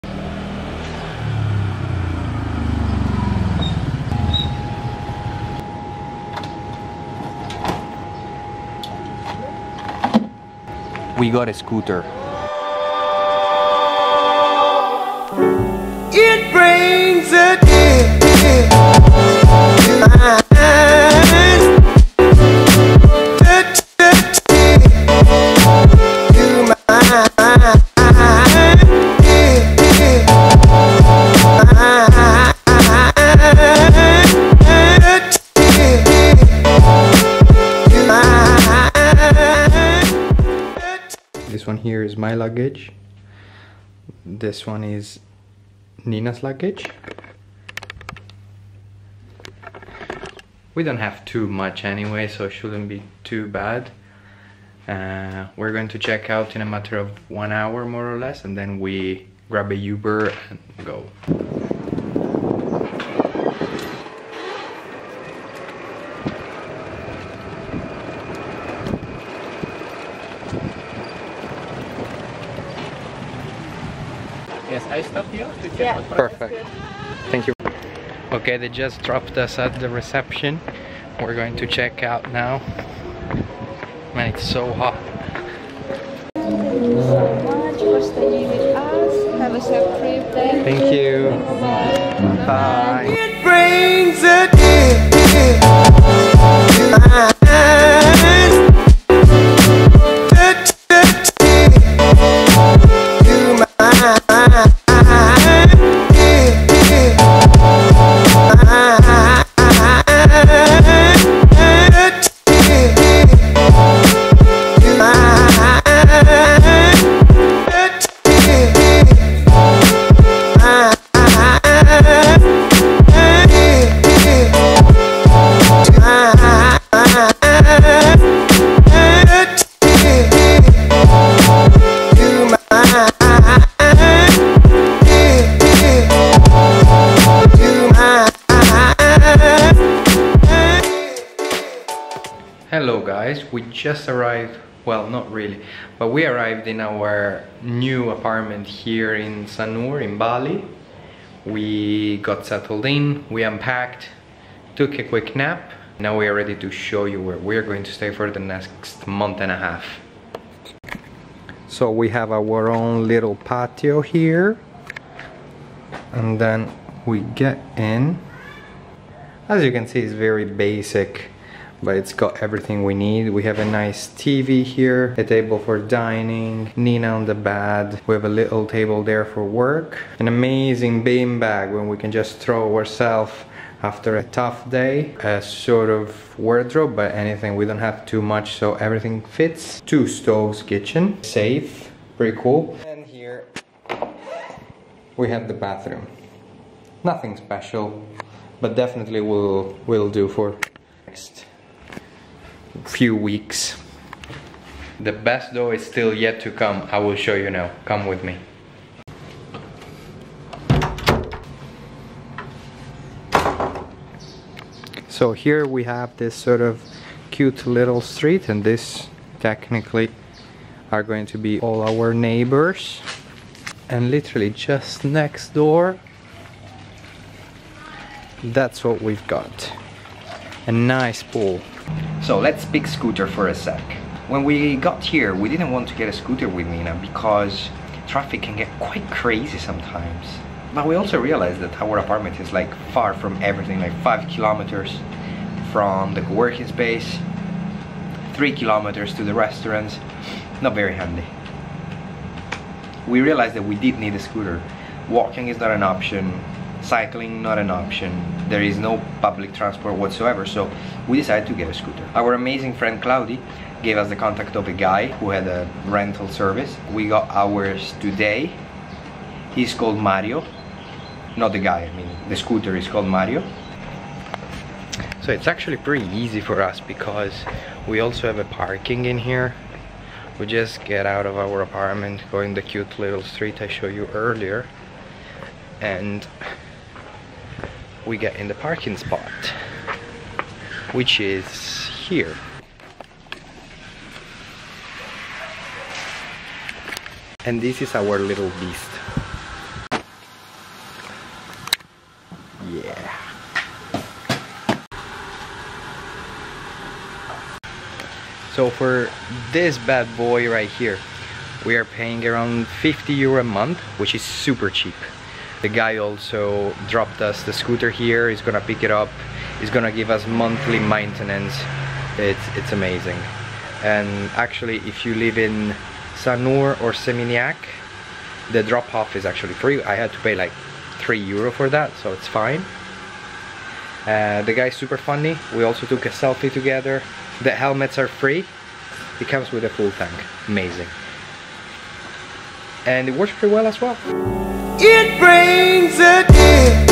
NEW APARTMENT TOUR AND RENTING A SCOOTER IN BALI - VLOG 03
After a few weeks in the Hotel, we're finally ready to go back into a "normal" life. We're getting in our new flat and renting a scooter as it's truly impossible to get around otherwise!

We're finally in Bali. We're gonna have two weeks of pure relaxation after two very tough months. We're staying in a beautiful resort in the south of the island